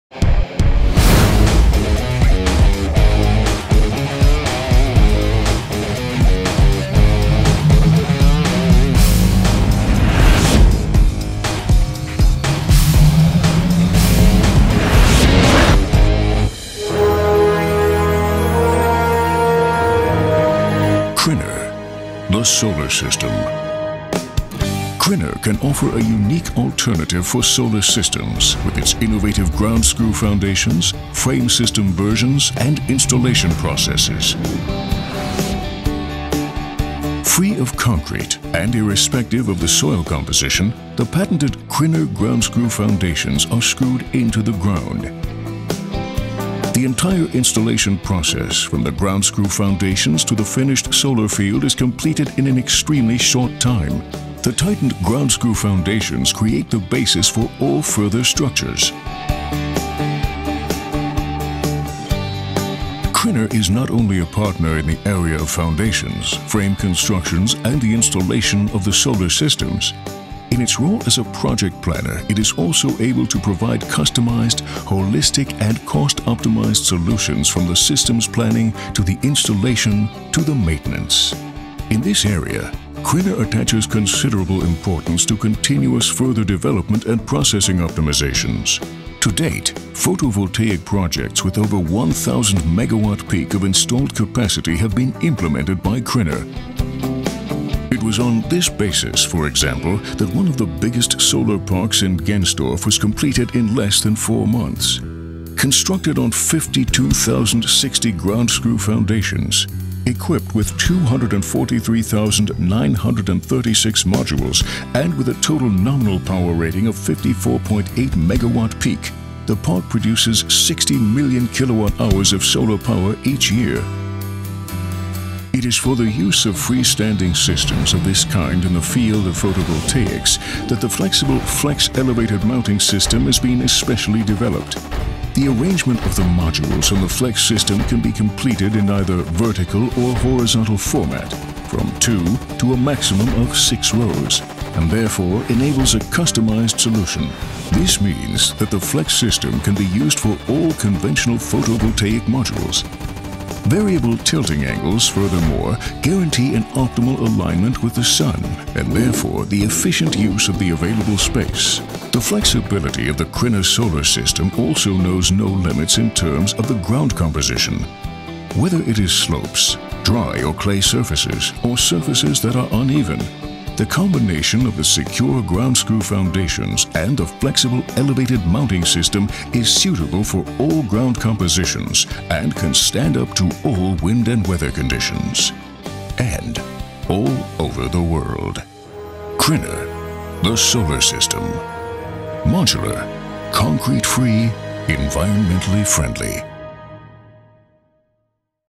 KRINNER The Solar System (English)
KRINNER Schraubfundamente GmbH, is a German company, which revolutionized the construction of solar plants with innovative solutions. Today more than 1,500 MWp of solar power have been installed worldwide. By combining our ground screws with the latest technology for PV substructures, such as slide-in systems, and our fleet of self-developed machines, KRINNERs Solar System guarantees an unrivalled easy installation and utmost cost efficiency.